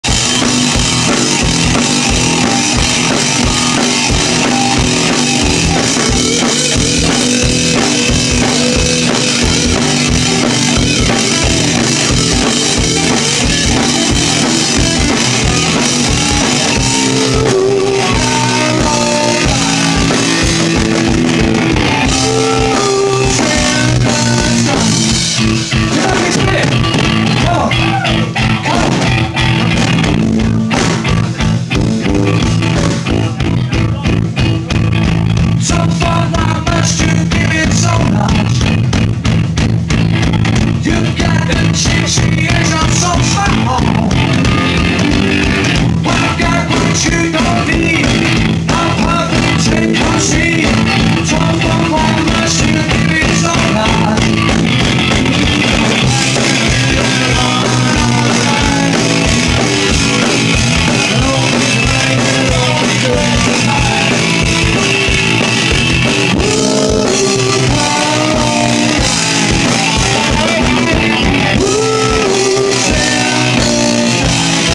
Officer Kicks (Tom Thumb), Water Rats Theatre
ROCK, of the newer wave kind. One of the UK's best live thrills, Officer Kicks at an early days show.